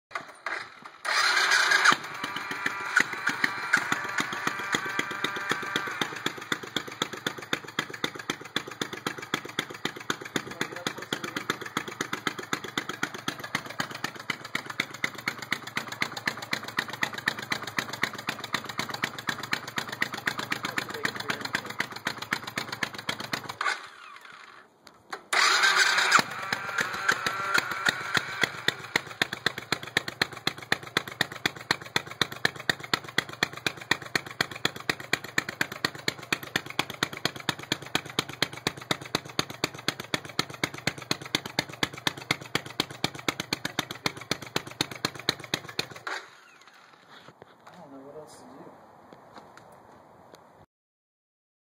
2001 Suzuki Savage
Runs with the throttle half way open. No response when turning the throttle. Always the same RPM's.